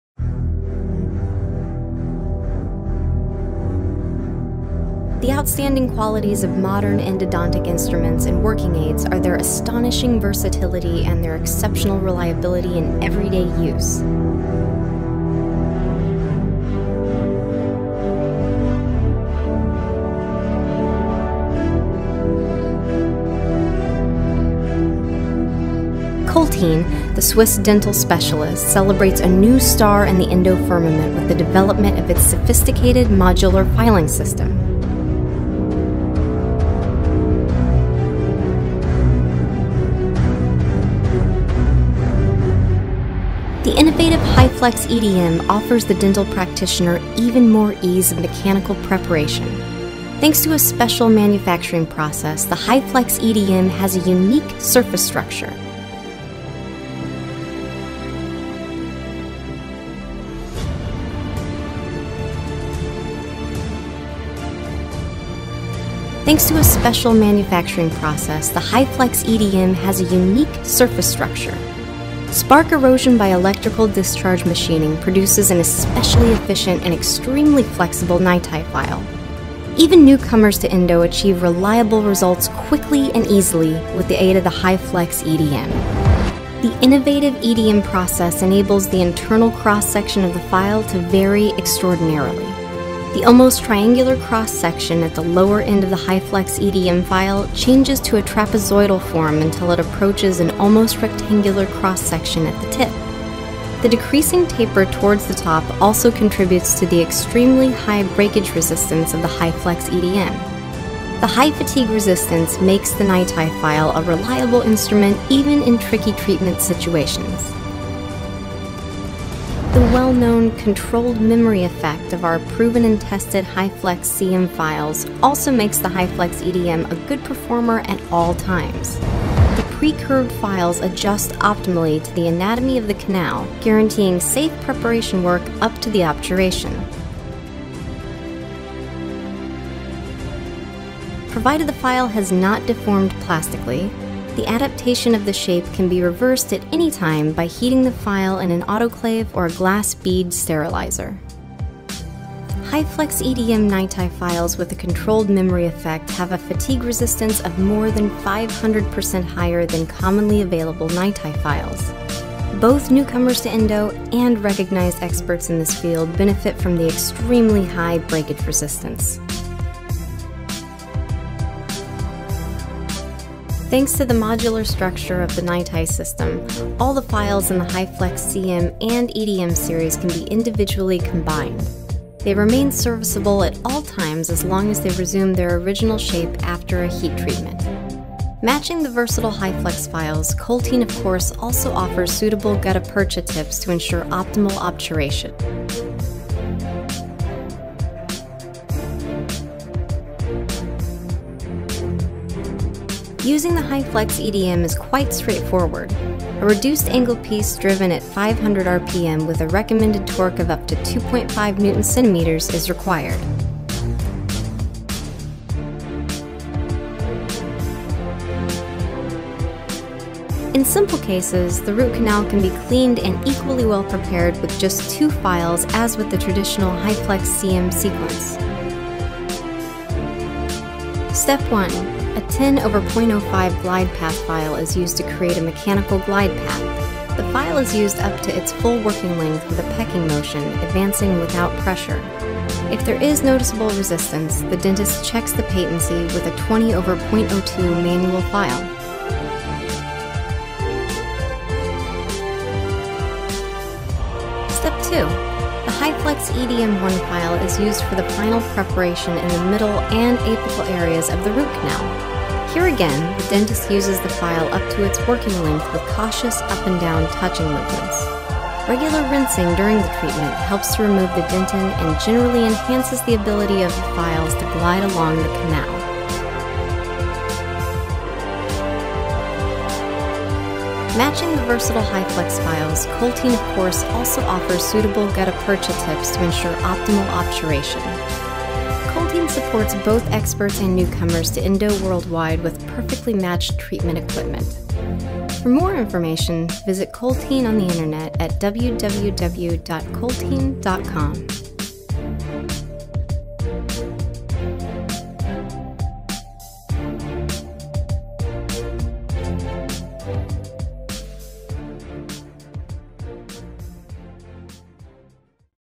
コルテン HyFlex EDM NiTiファイル
3Dアニメーションでコルテン社のNiTiファイル「HyFlex EDM」をご紹介いたします。ドクターたちのソリューションとなる、優れた形状記憶性・優れた根管追従性・優れた再生力の三拍子を併せ持ったNiTiファイルです。NiTiなのにプレカーブの付与が可能です。